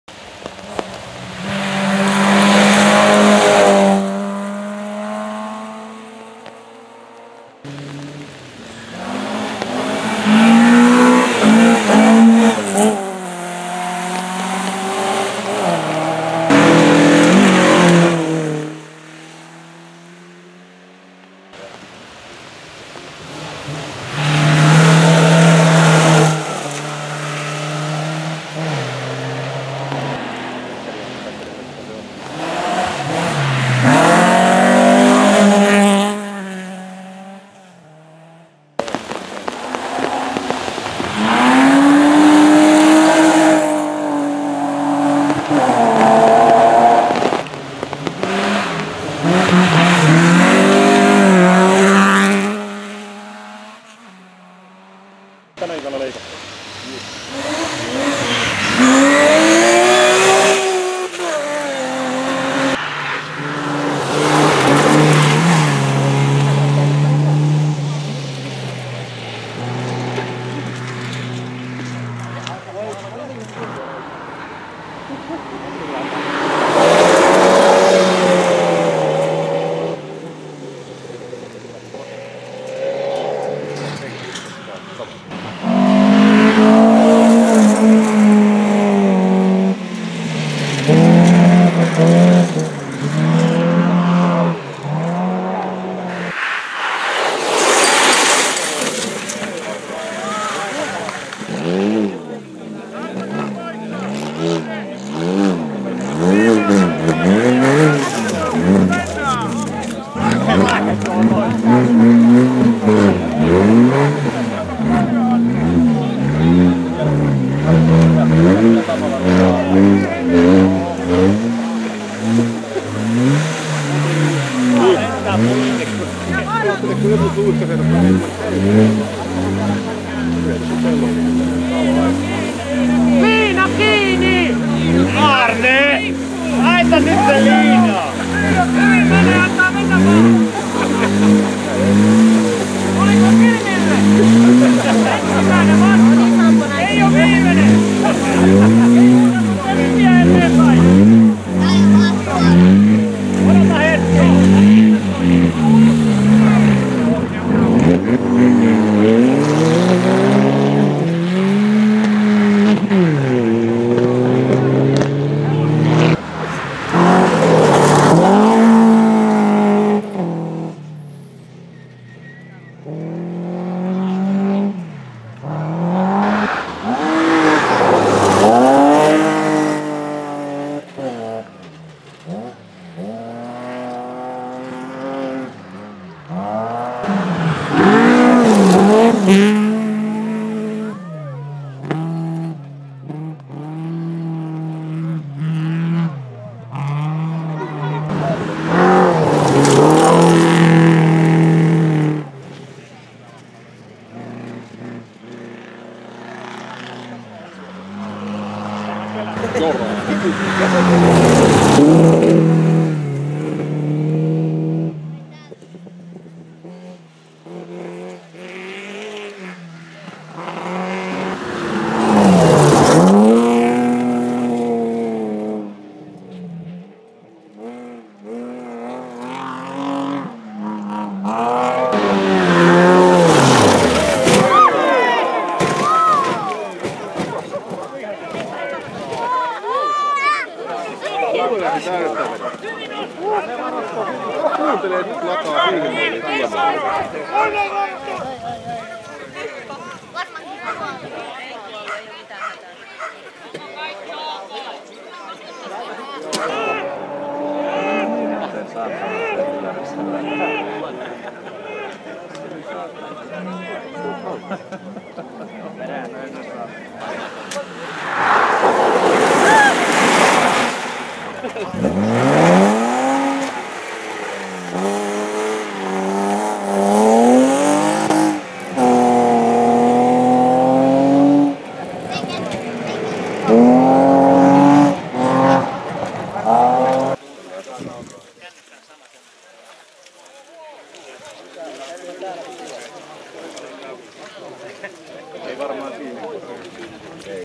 Ollin Sora Ralli 18.6.2016 | Historic Rally Trophy
Yksi kaato, pari suistumista ja likitilanteita Historic Rally Trophyn neljännestä osakilpailusta Jämsässä. EK1 ja EK4. One roll, few offs and close call from fourth round of the 2016 finnish Historic Rally Trophy at Jämsä. SS1 & SS4.